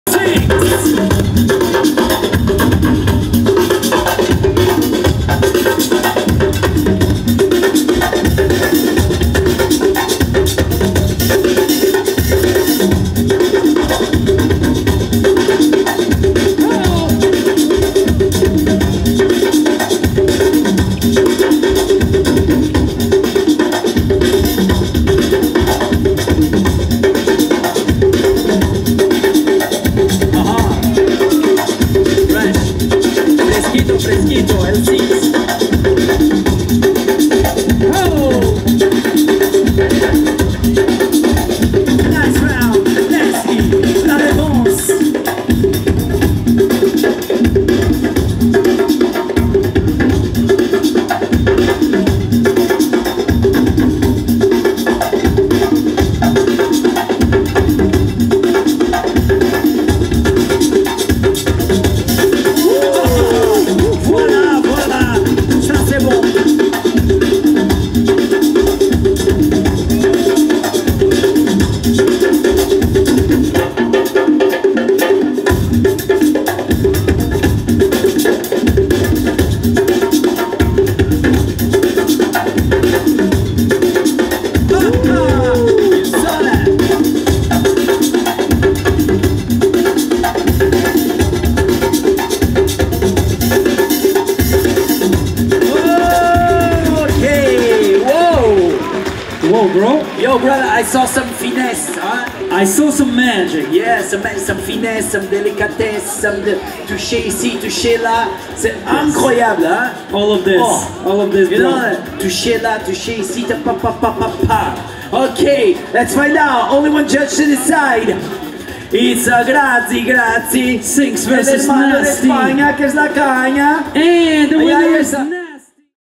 JBL Unbreakable 2021 - 1vs1 Bboy Top 32 Sinx vs Nasty
Filmed & edited by: LawkSam